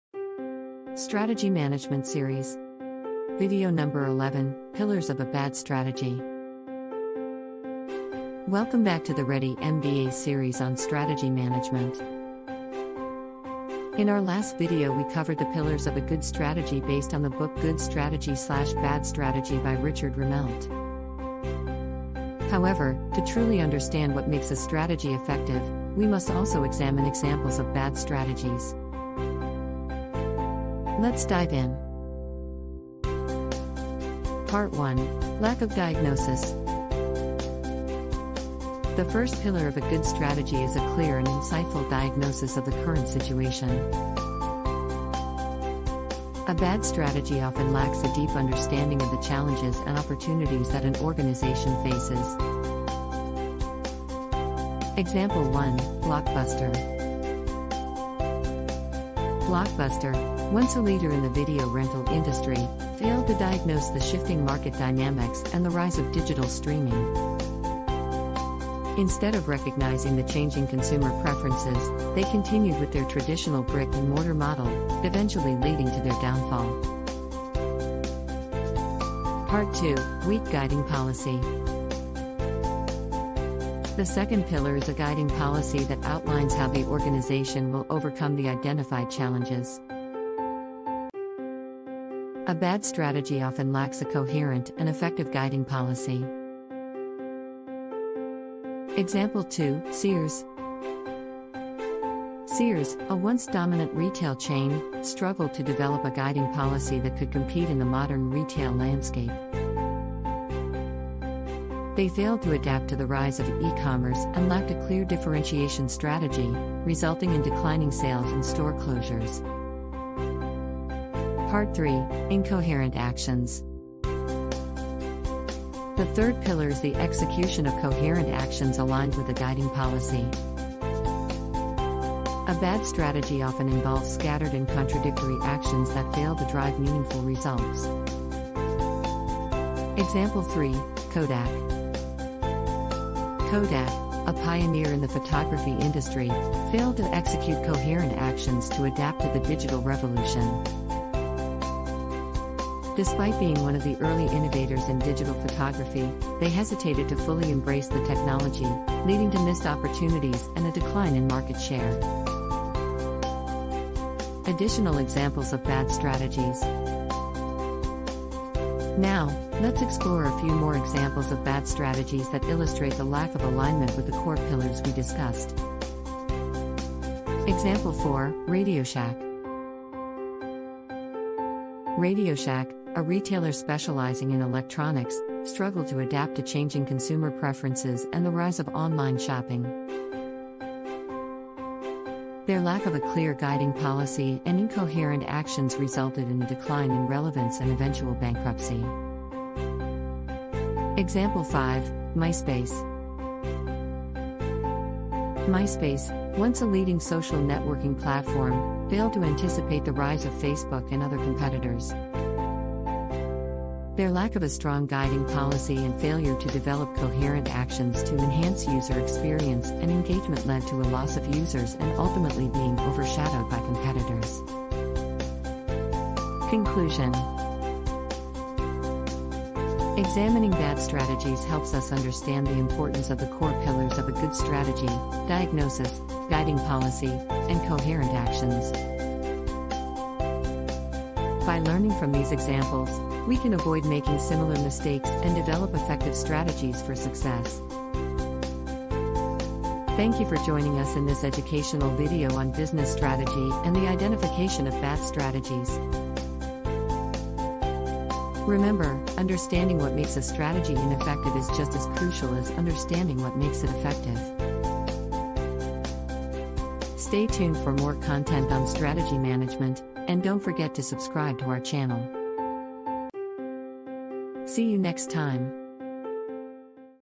Strategy Management - Pillars of a Bad Strategy (Video #11)
In our previous video we covered the pillars of a Good Strategy based on the book "Good Strategy/Bad Strategy" by Richard Rumelt.
However, to truly understand what makes a strategy effective, we must also examine examples of bad strategies.